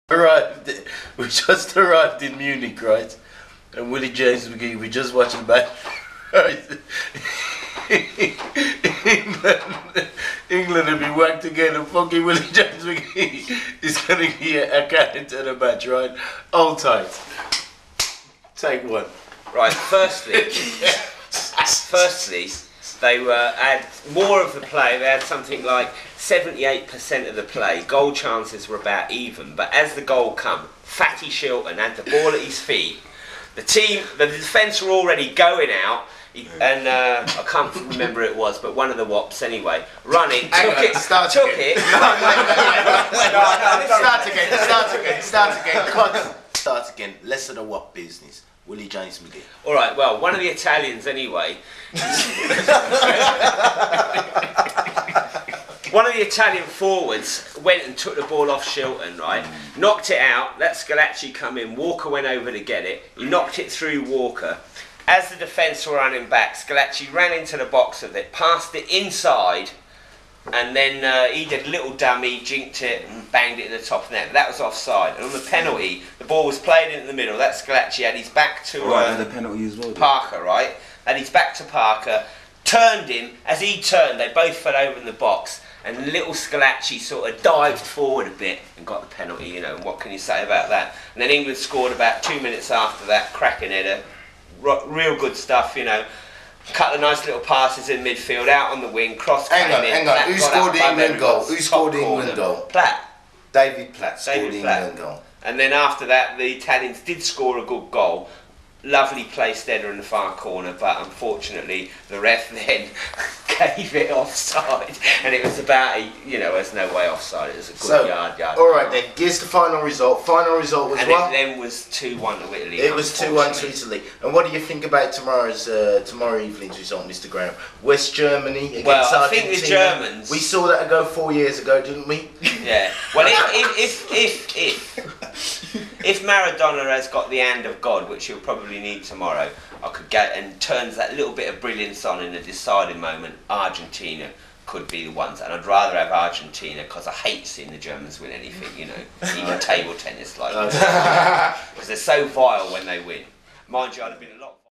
Street Noise - Trevor Taylor - MTV Tour 1991_Willie and Trev football talk
This was a very funny time with Trevor Taylor and Willie James McGhee, Bonnie and Eric XL Singleton. On Tour in Germany for MTV 1991.
Willie James McGhee explaining the football situation from that world cup in 1991. It was in the night after the Italy - England game (Semi Final).